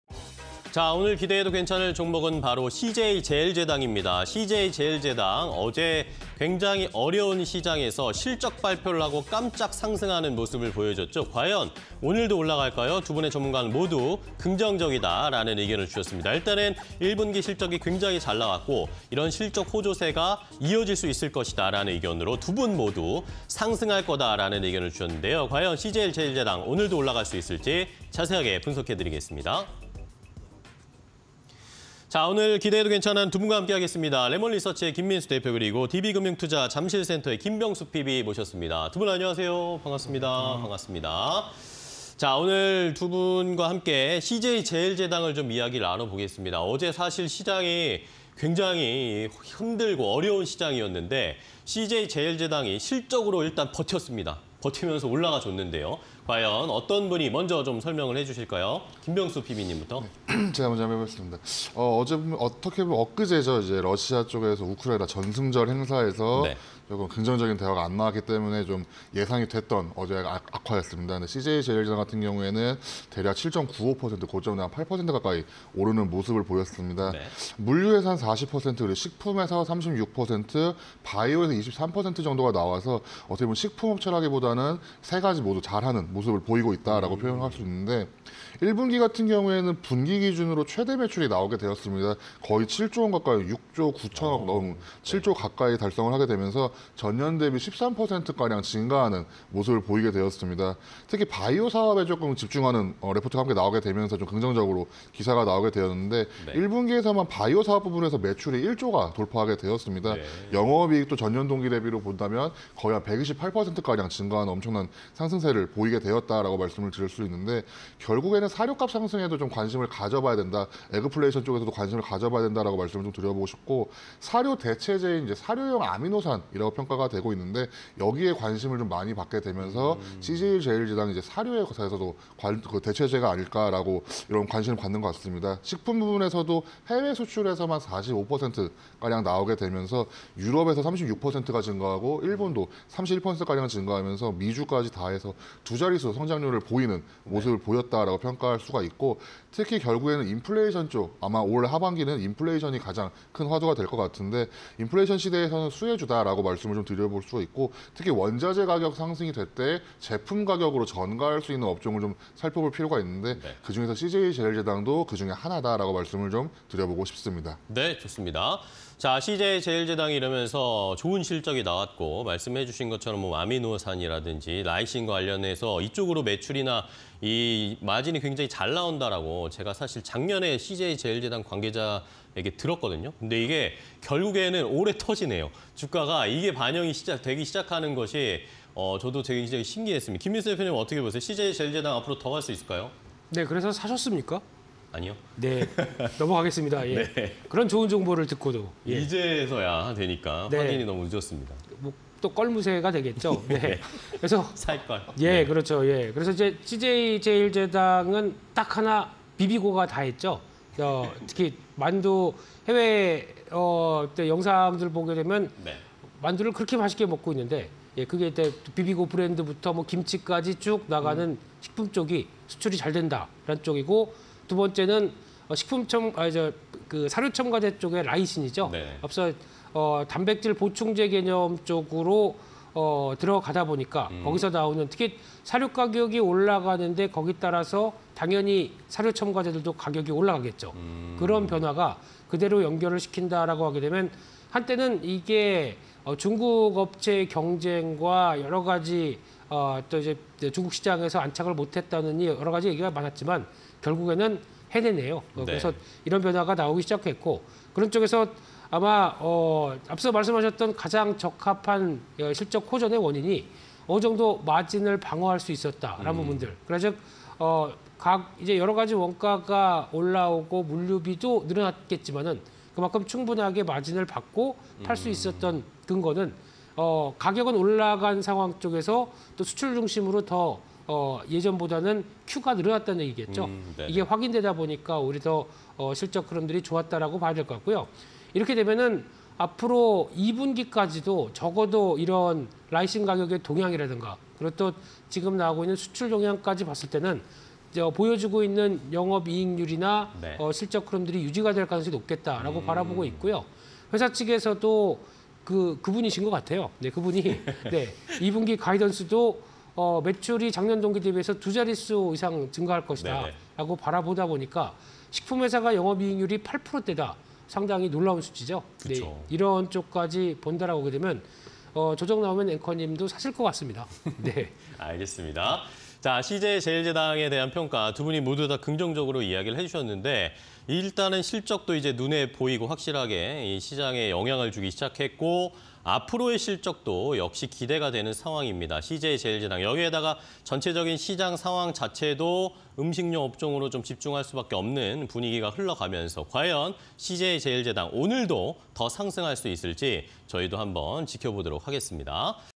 CJ제일제당(097950)... 실적 호조, 1분기 최대 실적_기대해도 괜찮아 (20220511)
CJ제일제당 더 간다
- 물류·식품·바이오 등으로 매출 구성
- 1Q 분기 기준 최대 매출 달성
- 바이오사업 부문 1Q 매출 1조 돌파
- 사료 대체제인 사료용 ‘아미노산’ 관심 증대
- 식품 부문, 해외 수출 비중 45% 차지
- 원자재 가격 상승, 가격으로 전가 가능

- 식품 및 바이오(라이신) 주력
- 1Q 매출 6.97조원, 영업익 4,357억원
- 국내외 가공식품 판매 +11%
- 가공 원가 부담… 판가 인상 기인해 마진 방어
- 바이오·곡물가 급등… 대체제 이익 기여
- 2Q 매출 YoY 기준 증가·영업이익률 8% 전망